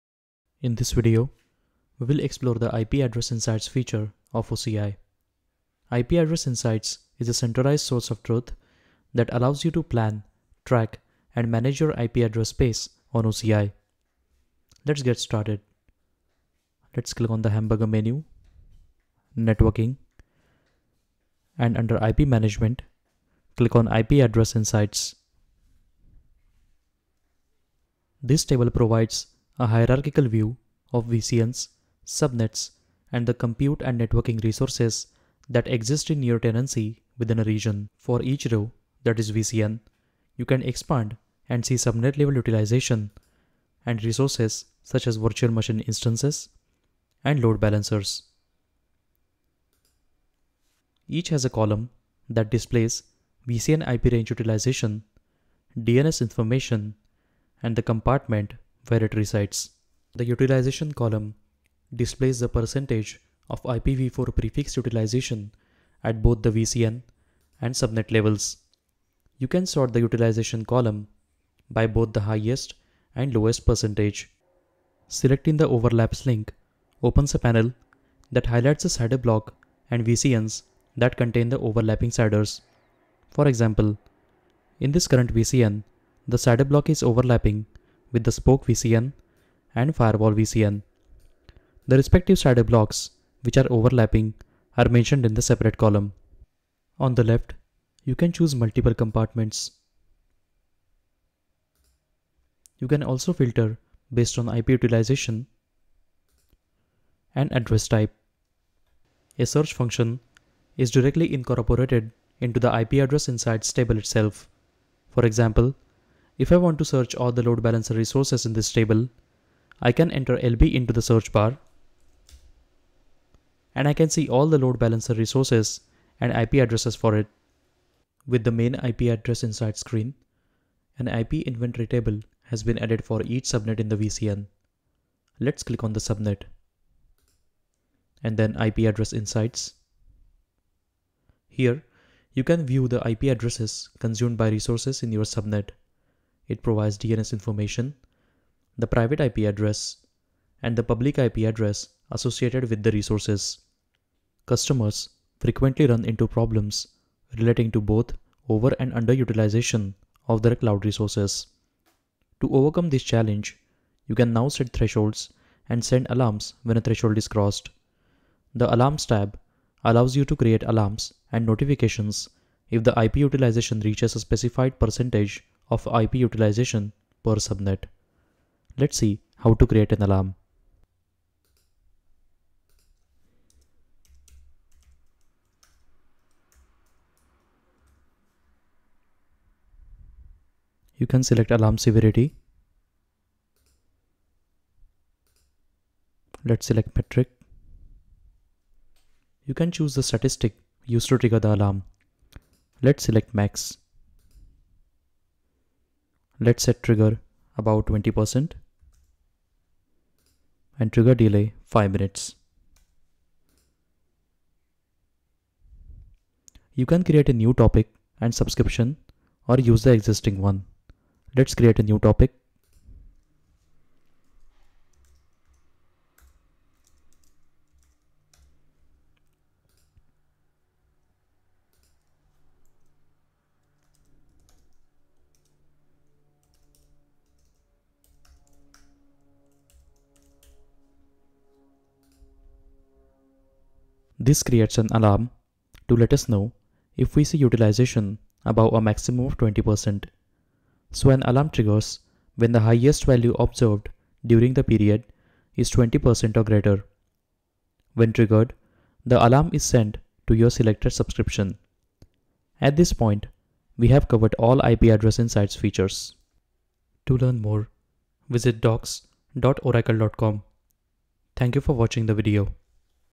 OCI IP Address Insights
This video demonstrates IP Address Insights feature on OCI.